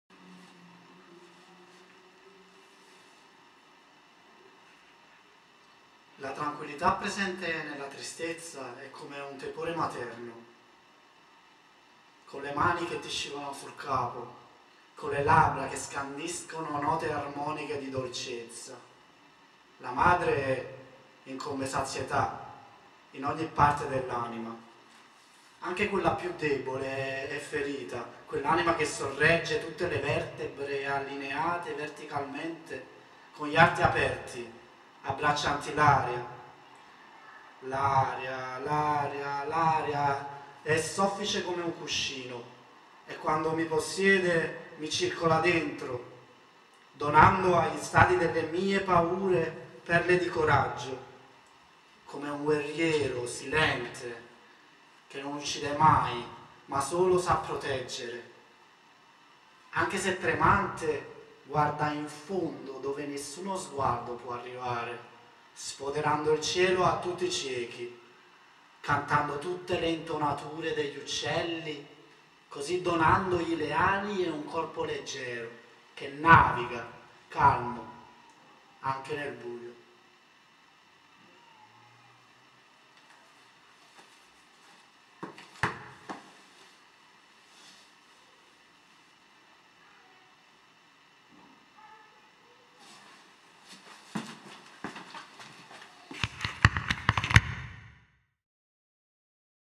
Poetry in a lock down "Aria"
During Cpoovid-19 lockdown the artist dedicates himself in the writing of poetry, thiis time in his own language.

Durante il lockdown l' artista Napoletano residente a Londra scrive e recita le sue poesie dal suo studio e giardino.